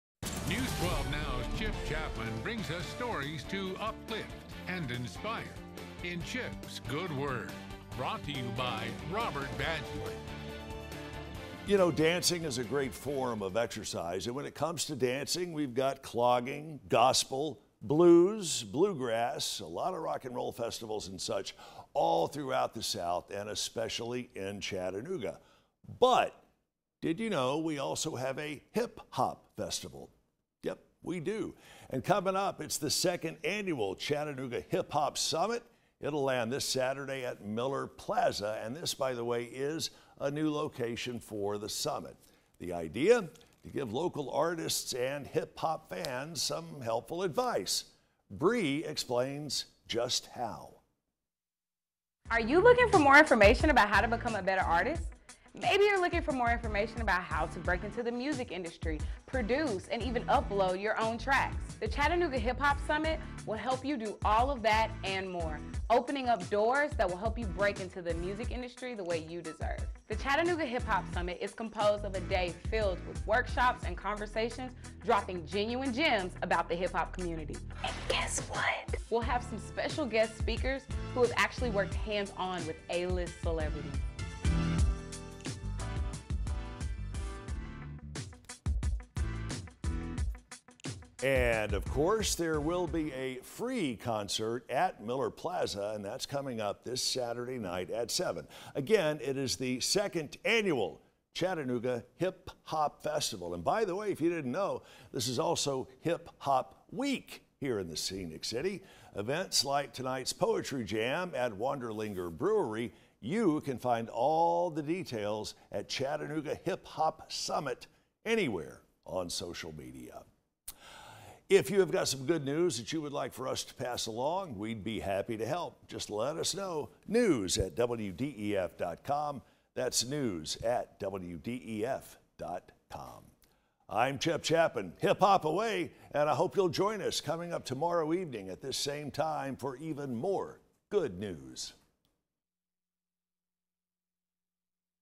Chip's Good Word: Hip Hop Summit
Chip shares details on the Chattanooga Hip Hop Summit on Saturday